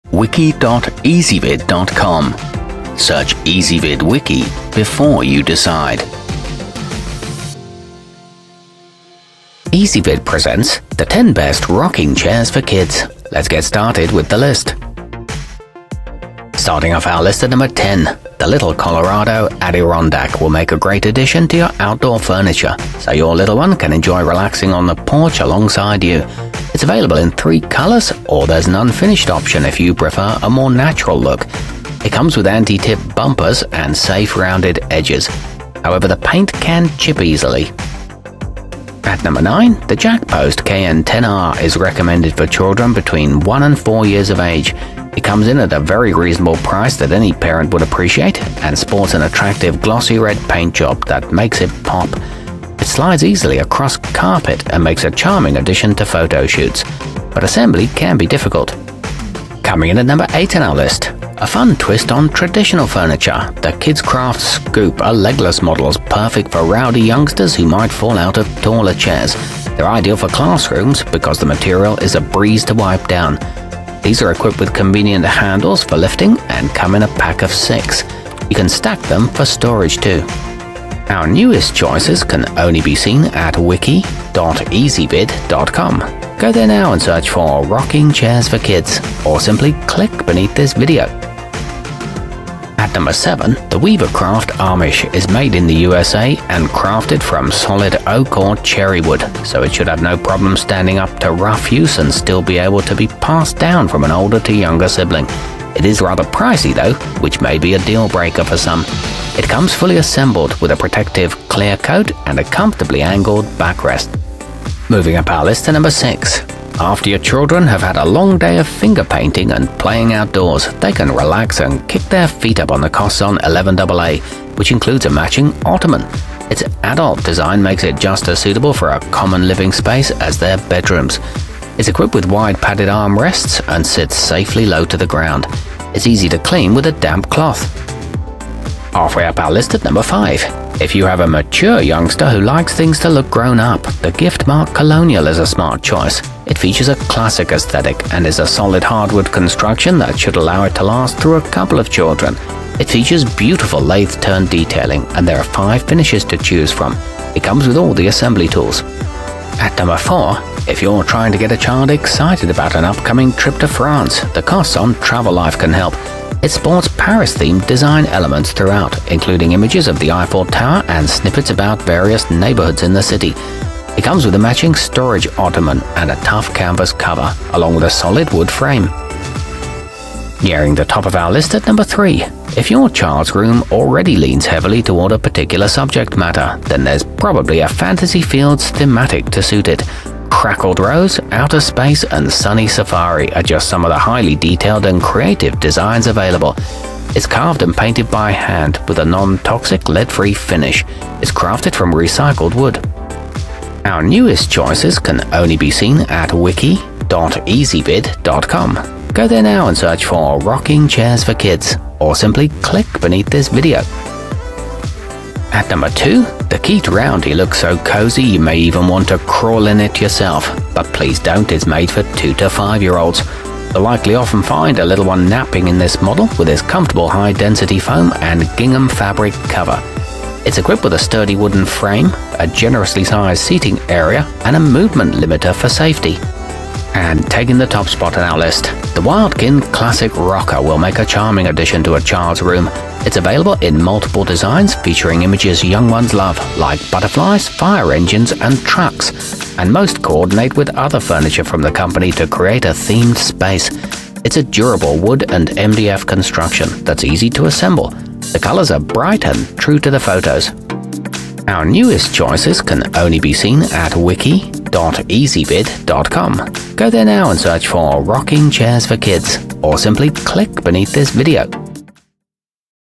10 Best Rocking Chairs For Kids 2019
Our complete review, including our selection for the year's best rocking chair for kids, is exclusively available on Ezvid Wiki.

Rocking chairs for kids included in this wiki include the fantasy fields thematic, weavercraft amish, costzon 11aa, wildkin classic rocker, gift mark colonial, kidskraft scoop, keet roundy, little colorado adirondack, costzon travel life, and jack-post kn-10r.